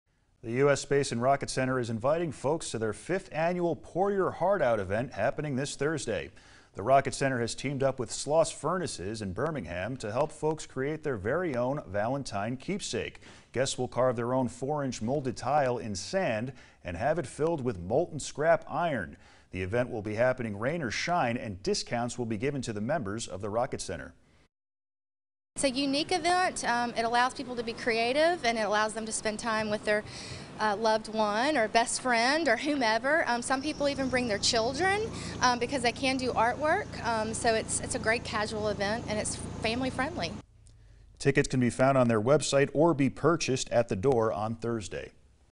Pour Your Heart Out event Thursday
The US Space and Rocket Center is hosting the "Pour Your Heart Out" event this Thursday.